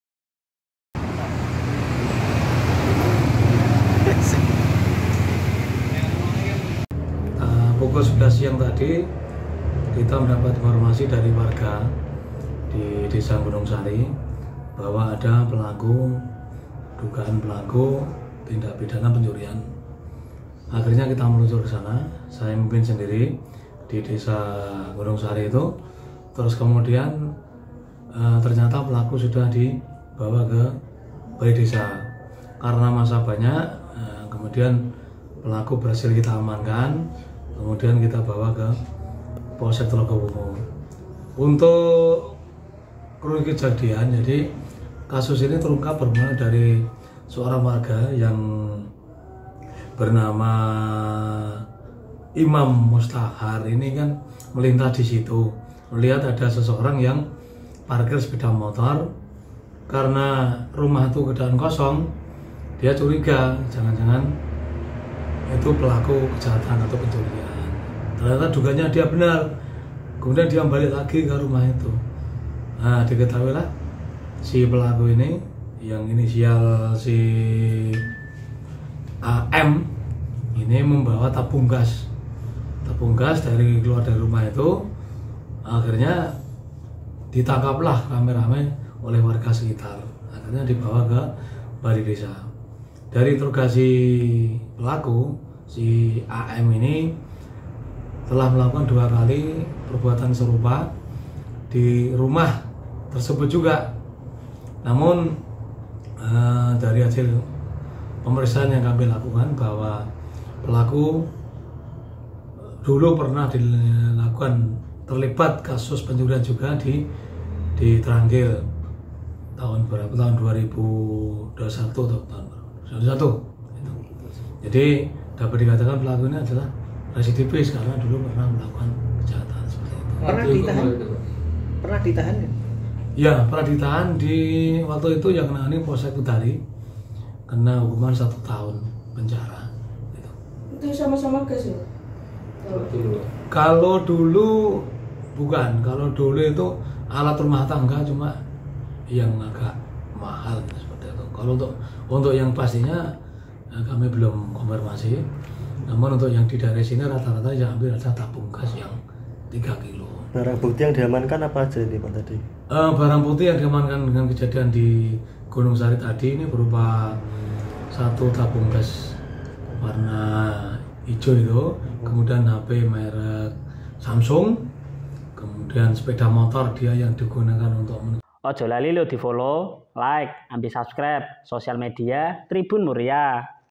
Apes! Ketahuan Maling Gas LPG, Pria di Pati Diarak Telanjang Dada ke Balai Desa
Pencuri tabung gas LPG melon di Kab Pati babak belur dihajar massa di Desa Gunungsari, Kec Tlogowungu, Kab Pati.

Pelaku pencurian itu ialah Ali Mansur (34), warga Desa Sambilawang, Kec Trangkil.

Residivis kasus pencurian ini apes karena aksinya kepergok warga. Dia jadi bulan-bulanan massa yang geram.